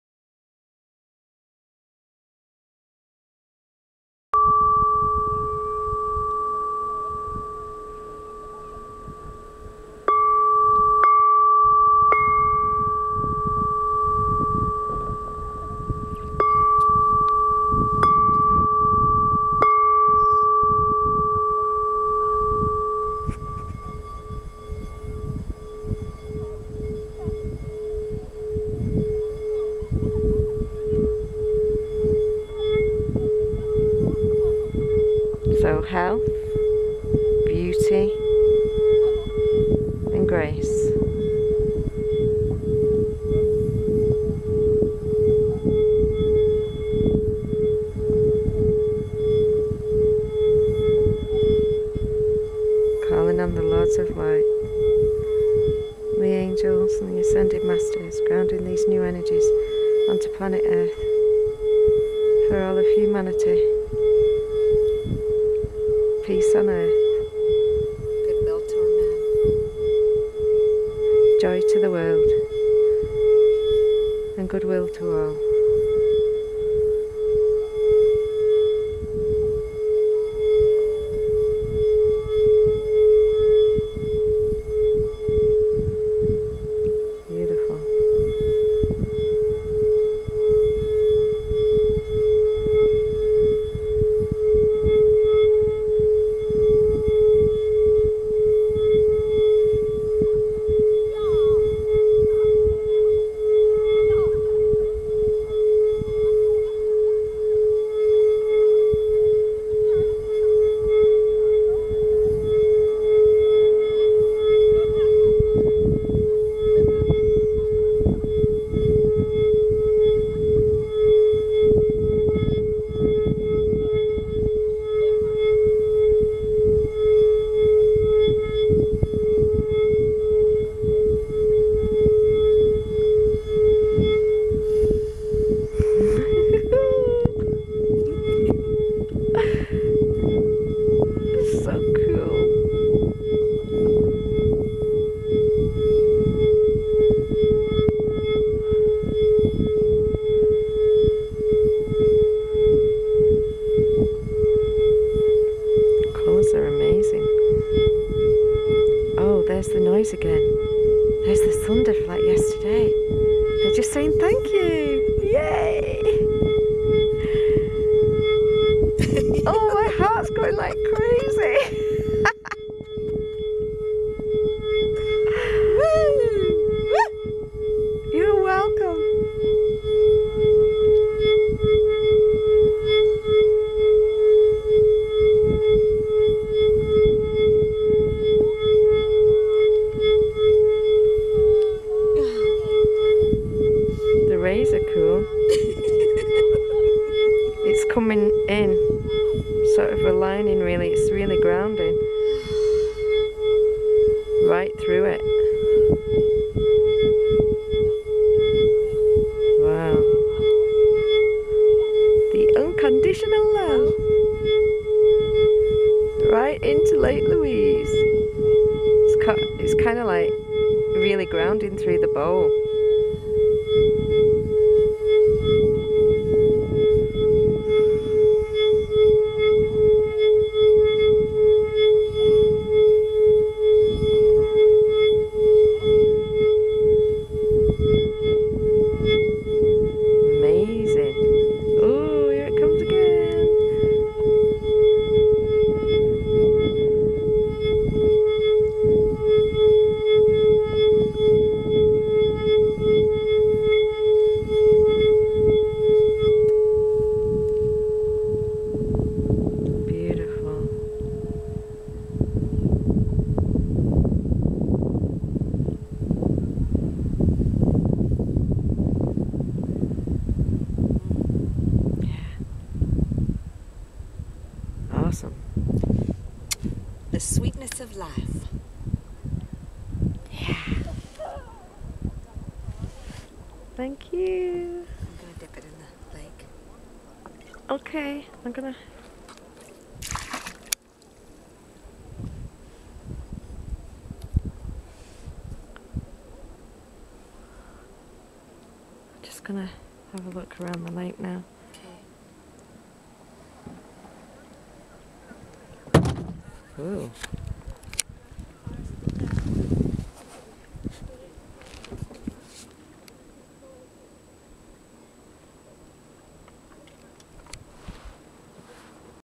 Lake Louise 2012
In 2012 we began our work. On our first trip we worked with the headwaters of the planet across USA and Canada. Here we are on Lake Louise. We had no idea at the time what we were doing. Just following our guidance